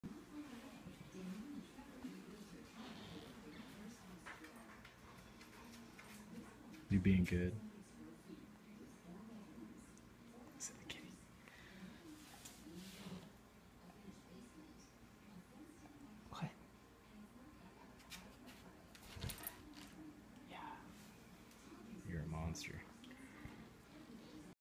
Lucas at home with the cats
Lucas, the Šarplaninac. Also good with cats!